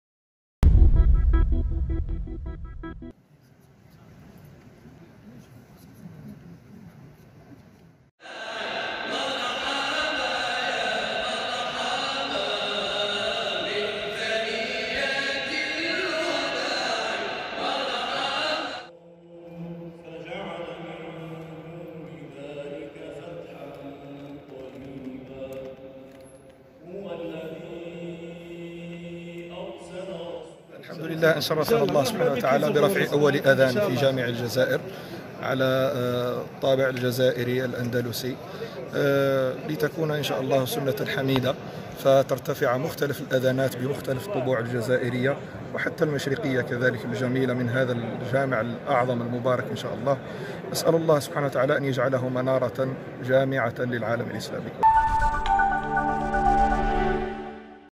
جامع الجزائر: افتتاح قاعة الصلاة بمناسبة ذكرى المولد النبوي الشريف
الجزائر(وأج) -  أشرف الوزير الأول عبد العزيز جراد على افتتاح قاعة الصلاة بجامع الجزائر بمناسبة الاحتفال بذكرى المولد النبوي الشريف، وذلك تحت الرعاية السامية لرئيس الجمهورية السيد عبد المجيد تبون.